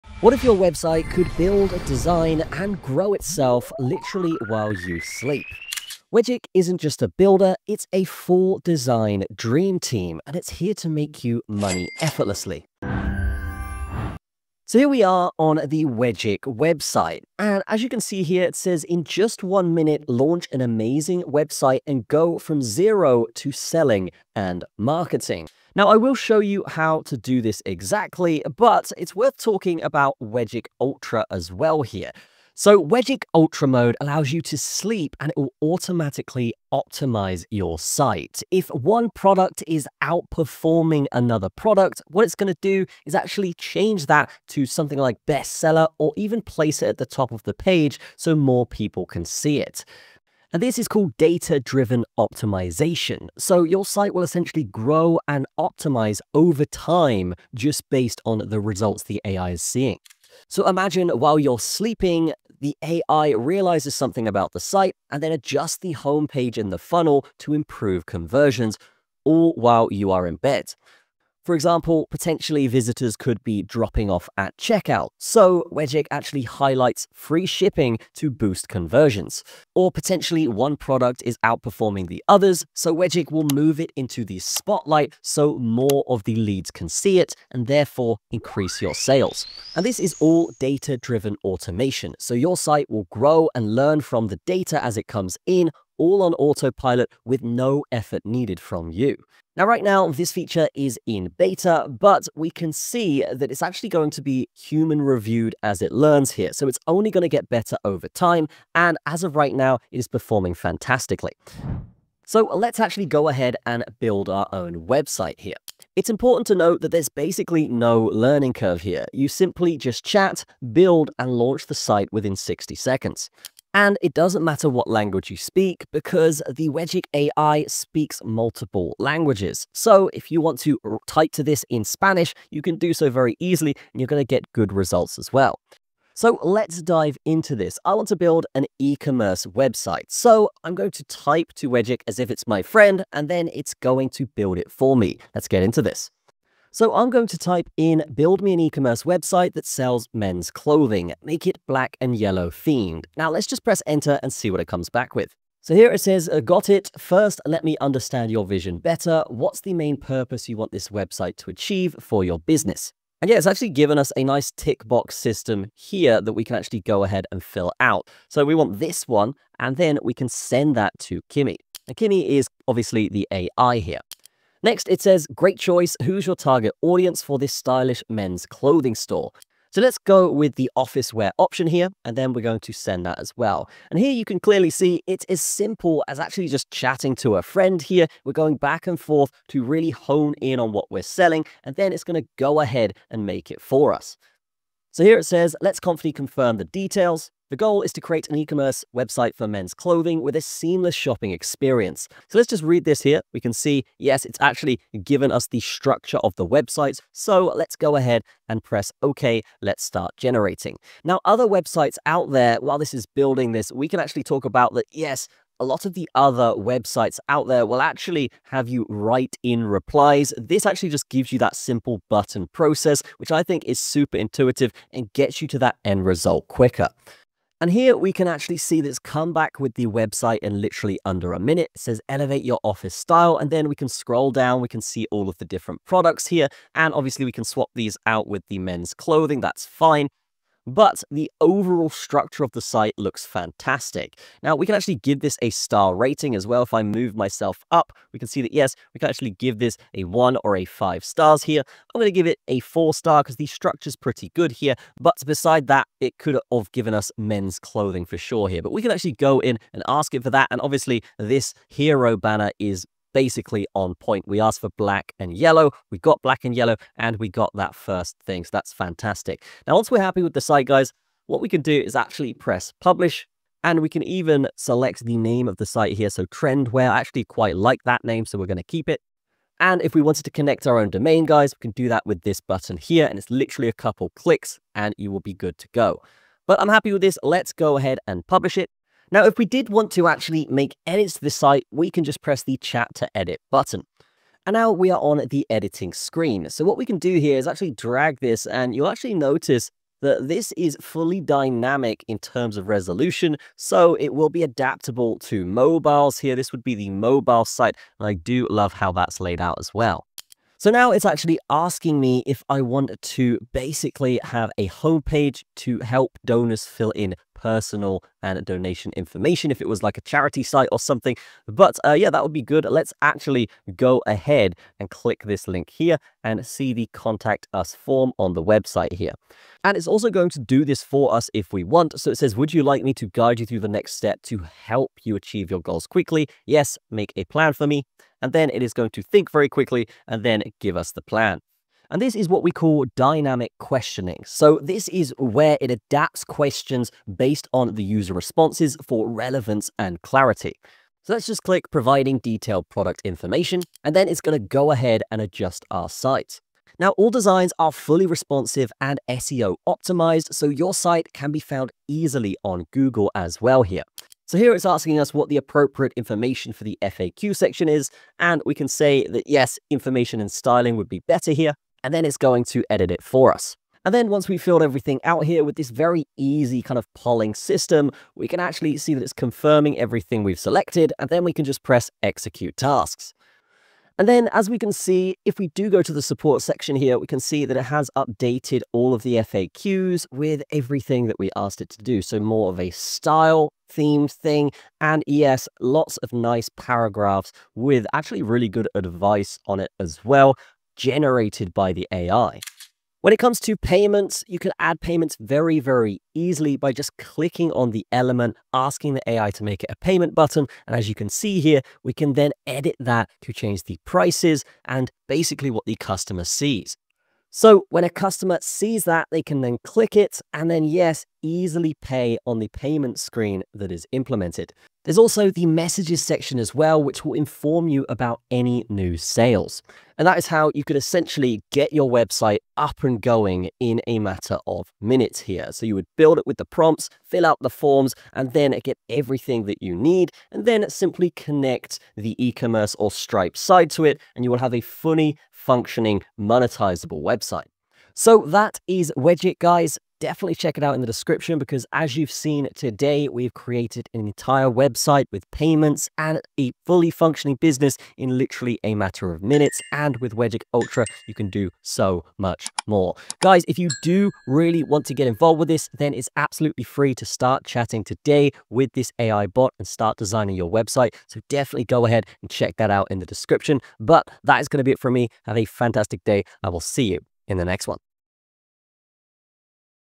AI Tools for Business: Build Websites While You Sleep
If you want a straightforward way to create a functional, well-designed website without coding or design skills, Wegic can help. This AI-powered builder allows you to set up your site quickly and make adjustments with minimal effort.

Key features of Wegic:
Build and launch a website in about a minute.
Wegic Ultra Mode can monitor and improve your site automatically over time.
Supports e-commerce with payment options and order notifications.
Multi-language capability for international audiences.
Responsive designs that adapt to mobile, tablet, and desktop.
Simple, chat-based interface for building and editing.

Wegic can be useful for small business owners, freelancers, or anyone who wants a website without spending time on complex setup or ongoing technical maintenance.

CHAPTERS:
00:00 - What is Wegic
00:32 - Wegic Ultra Mode
01:51 - Building a Website with Wegic
07:27 - Get Started with Wegic